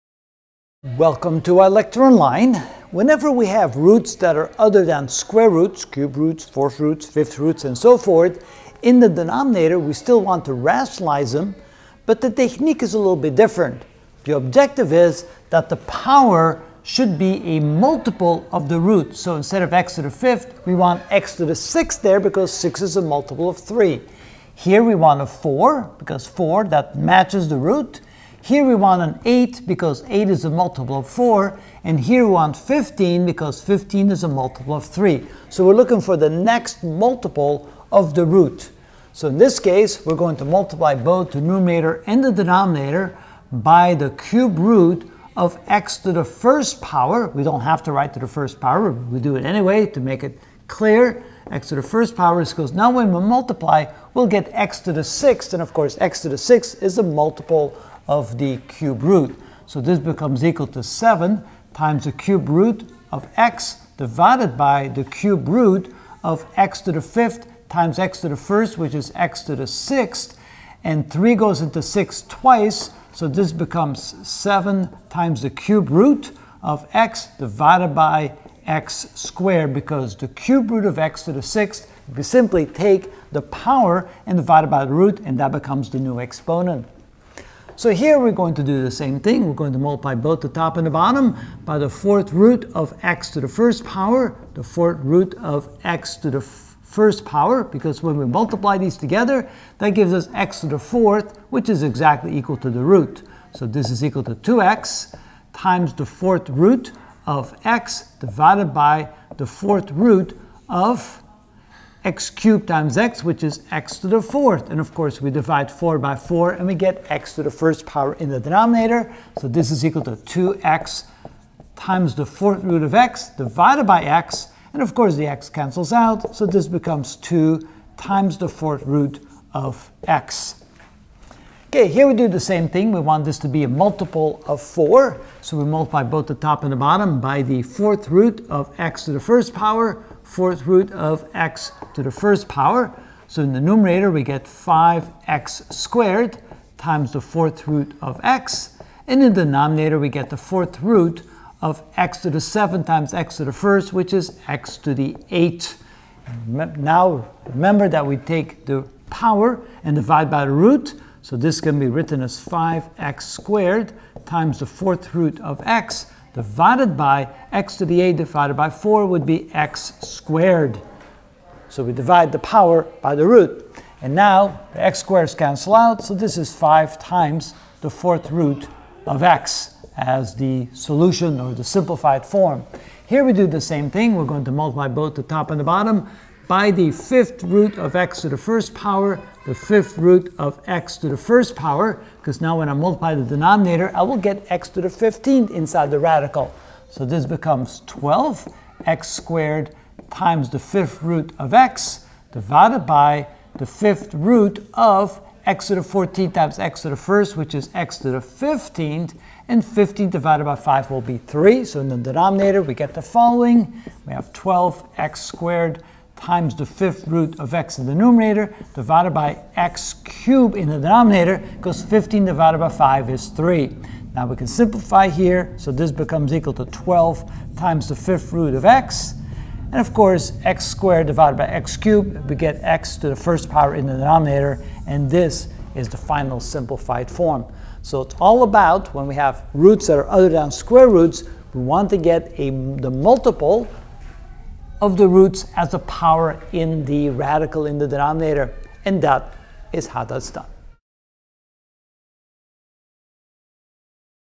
Algebra - Ch. 20: Simplifying Radical Expressions (11 of 15) Example Set #7 Rationalize Denominator
We will simplify 3 examples radical expressions by rationalizing the denominator. Example Set #7